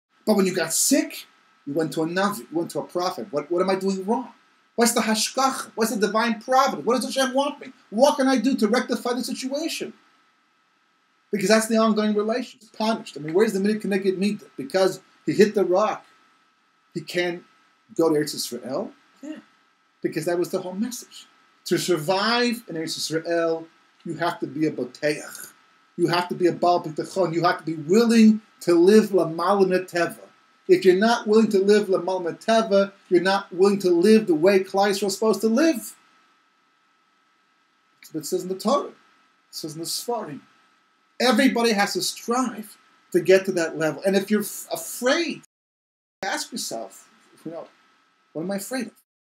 Parashas Chukas, Part2
Hitting the rock closed the door on the era during which a single person could bring about the Final Redemption. The question is, why was this episode so significant?